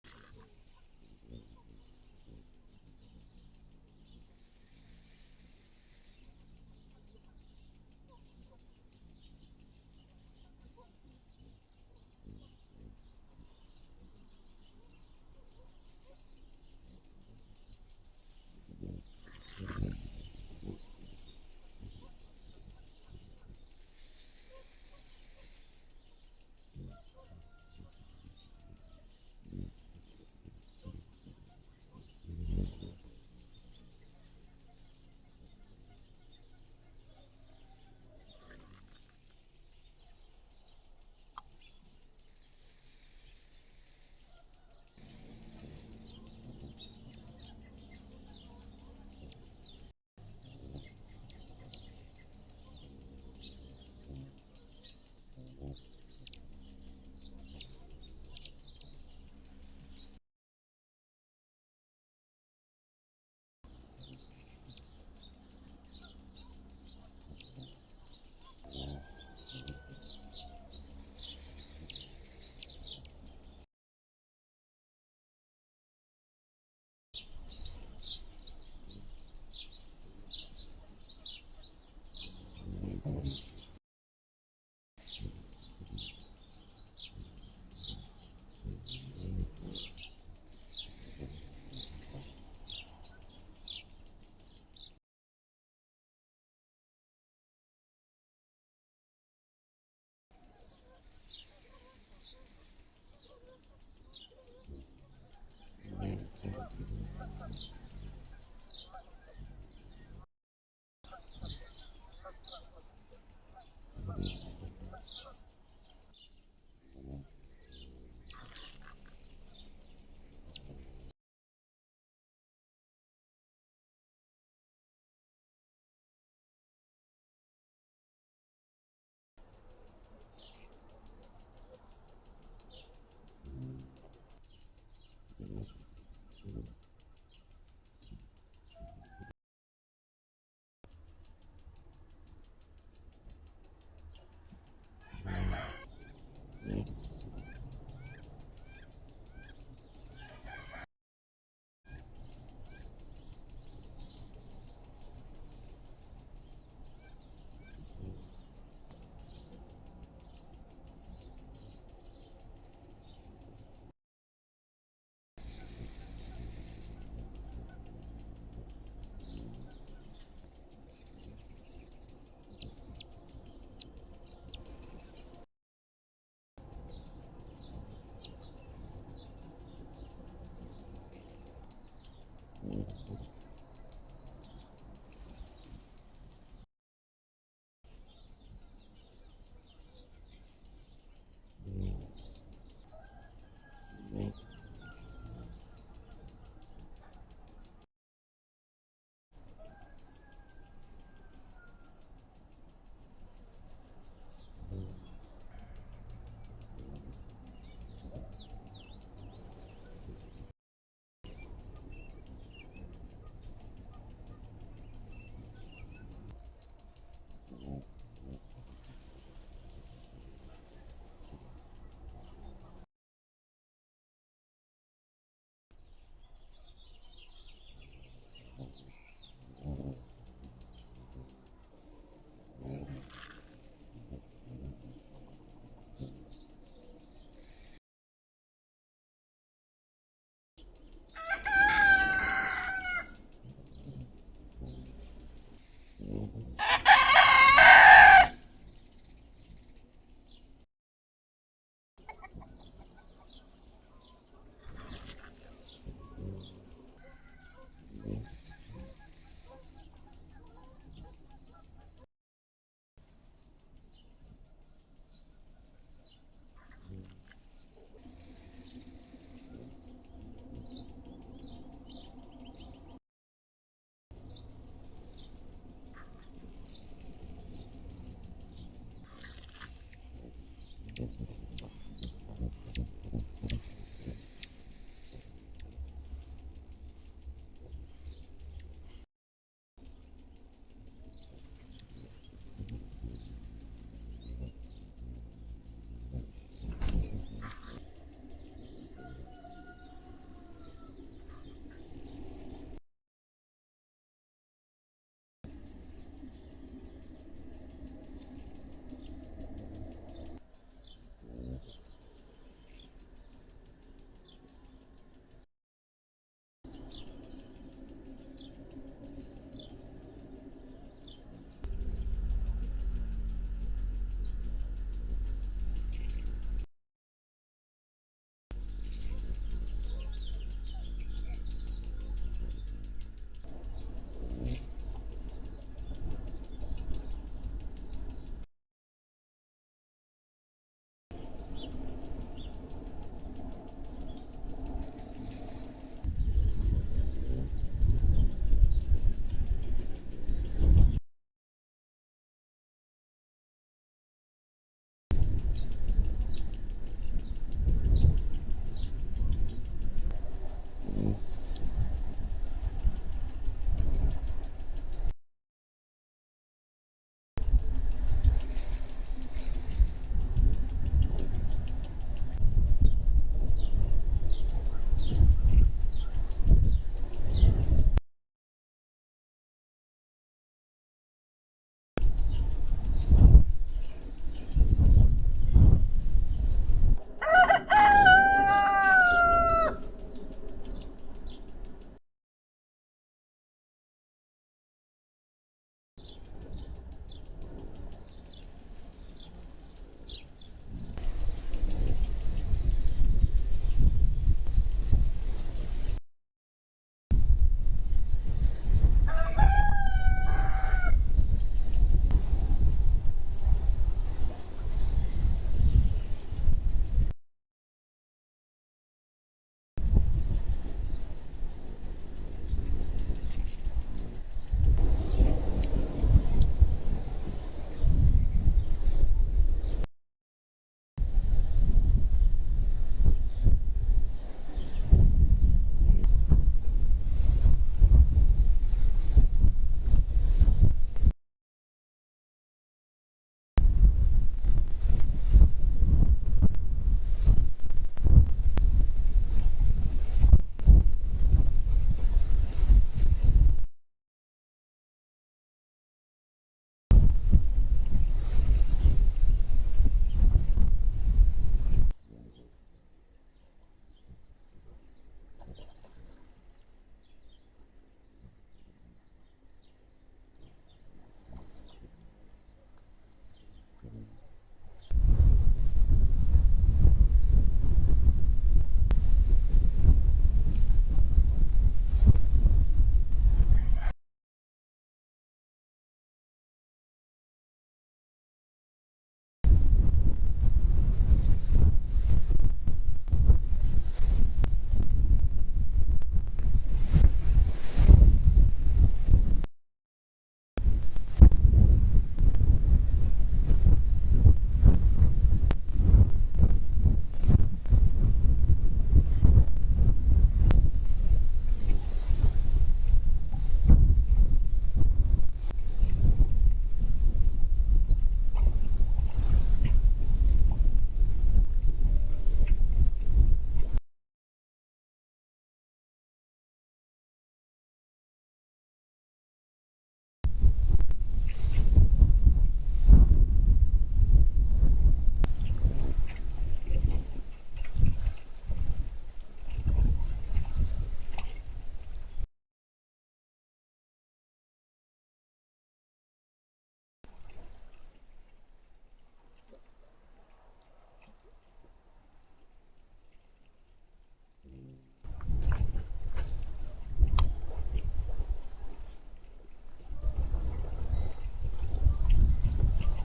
Anna's, Calliope, Rufous, and Black-chinned Hummingbirds - May 6, 2021
Black-chinned male made an appearance today. Sunny, windy, high of 77F.